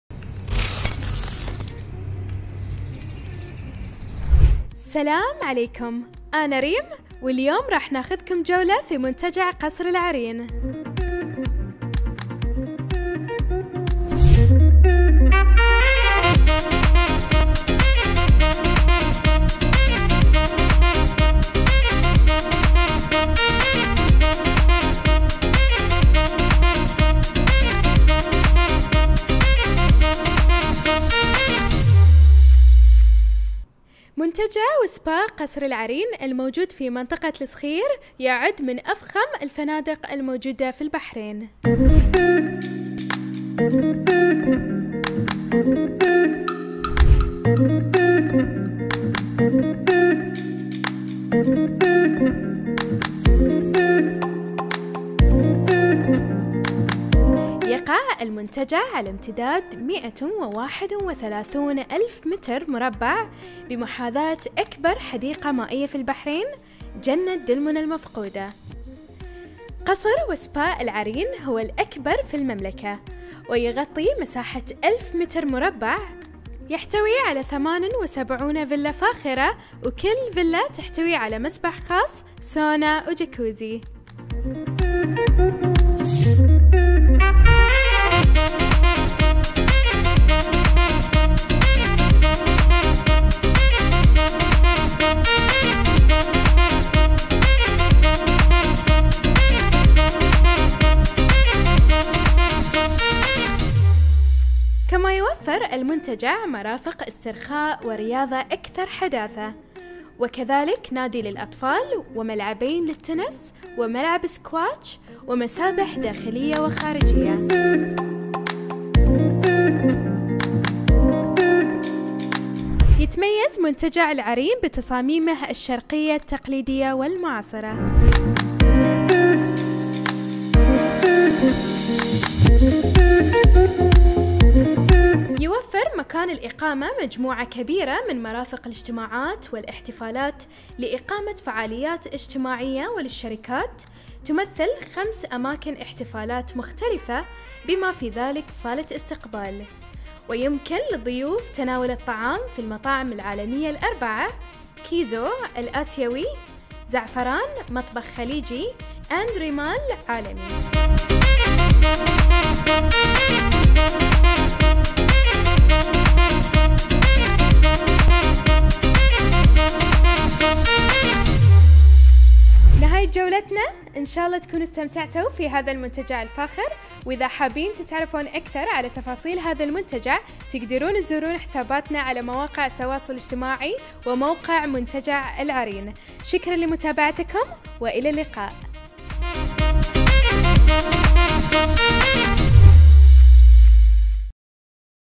Al Areen Palace and Spa by Accor| Al Watan News Coverage
Take a tour of our property with TV Presenter Reem from Al Watan News.